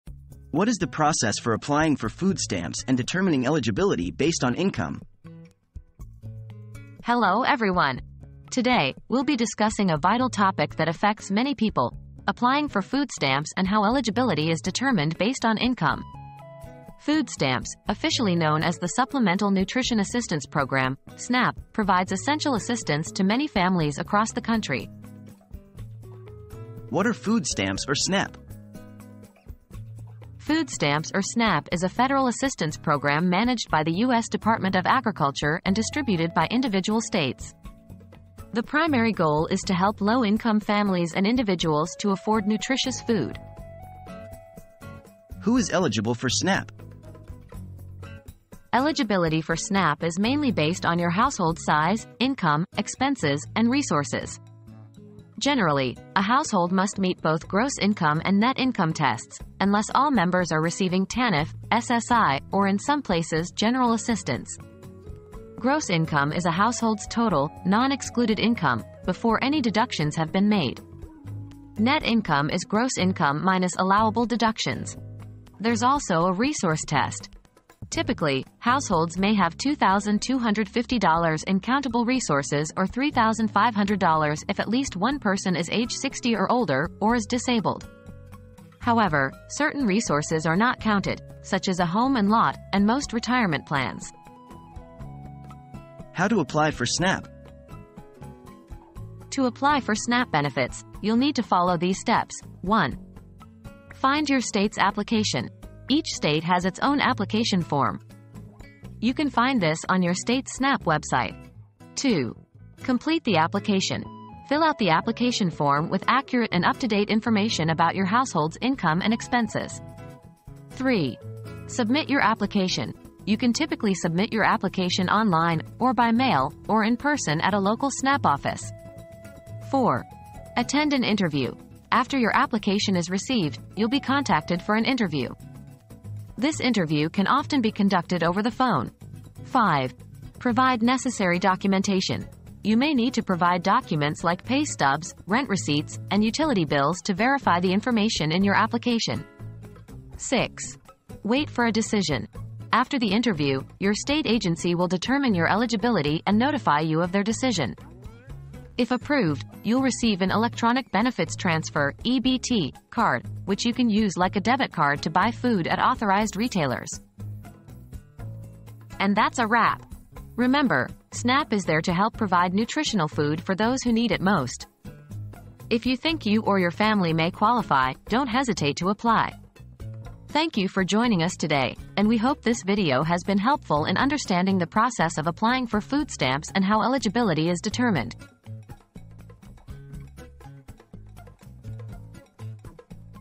What is the process for applying for food stamps and determining eligibility based on income?
Demystifying Food Stamps: Application and Eligibility Process • Demystifying Food Stamps: Application and Eligibility Process. Learn how to apply for food stamps and determine eligibility based on income. Find out who qualifies for SNAP and the steps to follow for applying. Watch now to understand this vital assistance program!

00:00 • What is the process for applying for food stamps and determining eligibility based on income?
00:26 • What are Food Stamps or SNAP?
00:45 • Who is Eligible for SNAP?
01:36 • How to Apply for SNAP?